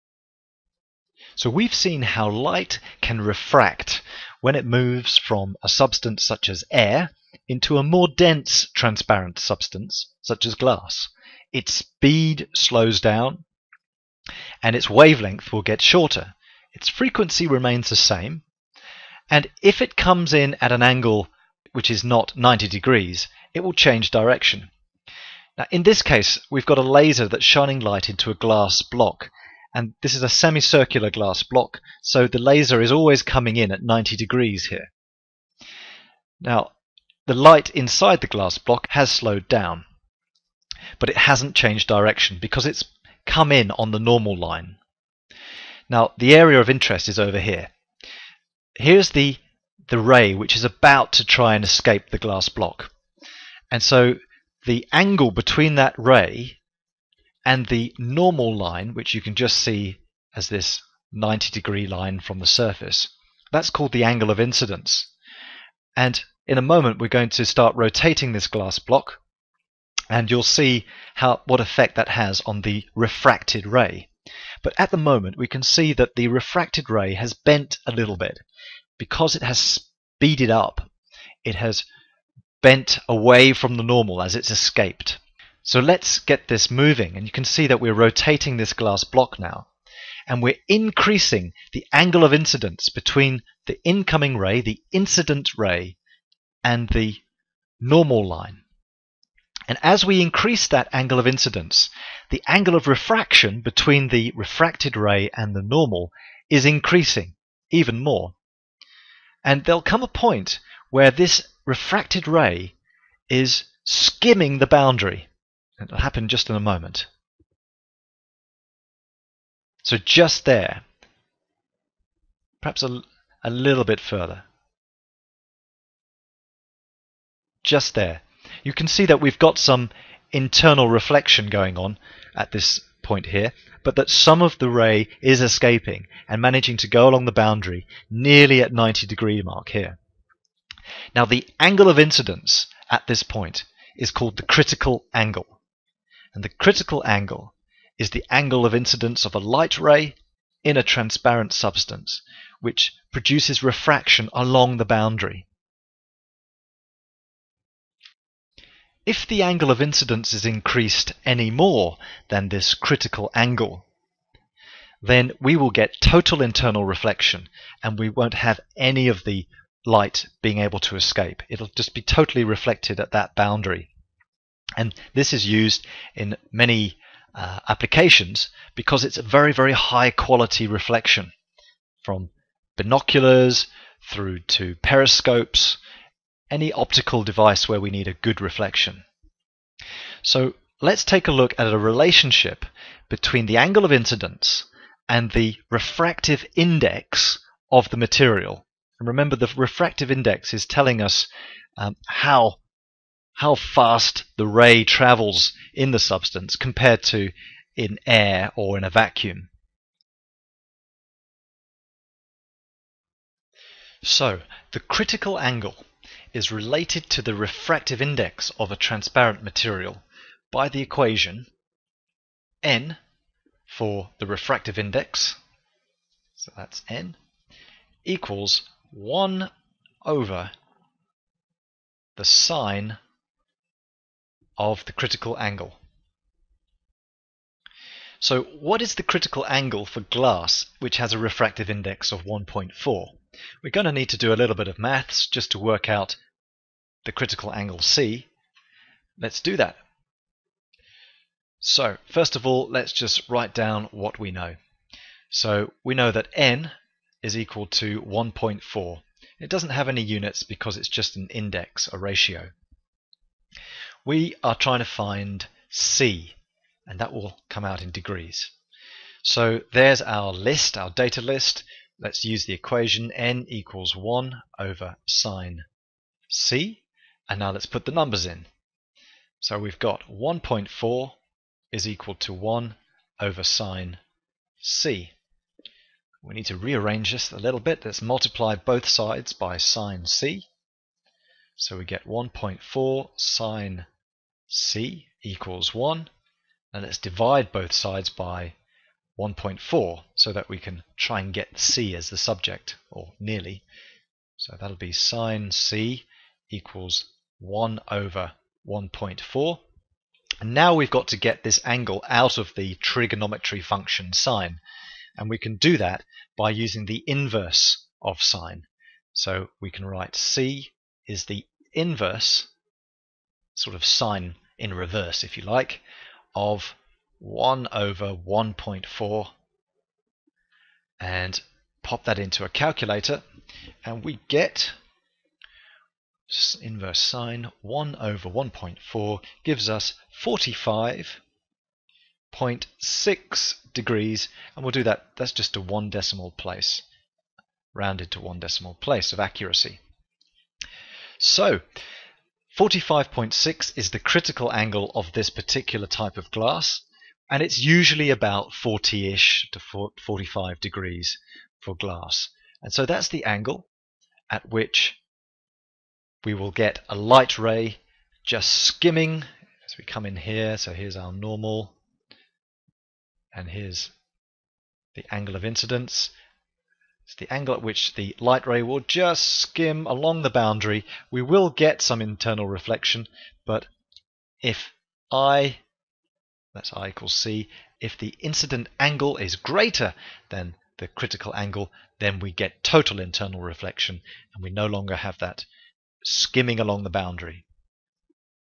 Critical Angle and Refractive Index - Equation & Calculation - GCSE Physics
The Critical Angle, C, is related to the Refractive Index, n, of a transparent material by the equation:
n = 1 / sin(C)